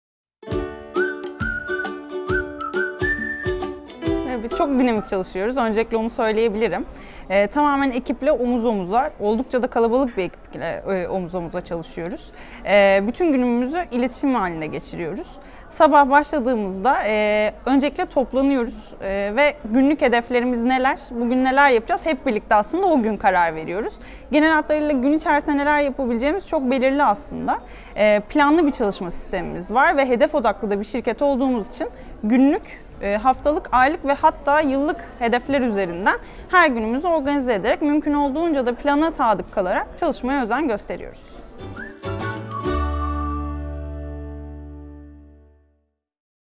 Pronet İnsan Kaynakları Departmanı'nda bir gün nasıl geçiyor?
Pronet İşe Alım Müdür Yardımcısı Nil Üngeldi Anlatıyor!

Pronet İnsan Kaynakları Departmanı'nı ve nasıl projeler üzerinde çalıştığınızı anlatır mısınız?